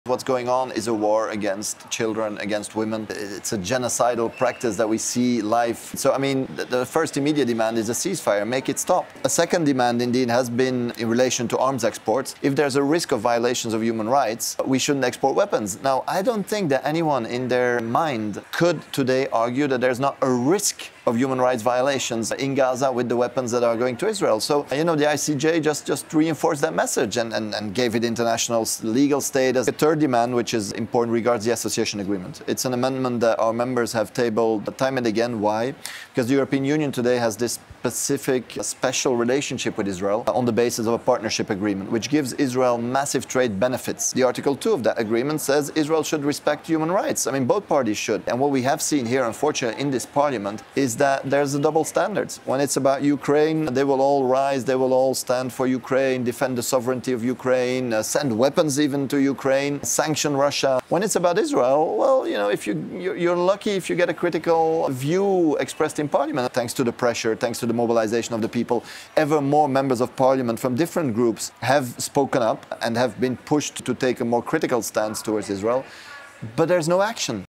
Anyone in their mind would not send arms to Isreal right now.
MEP Marc Botenga @ptbbelgique , 28/02/2024, European Parliament, Strasbourg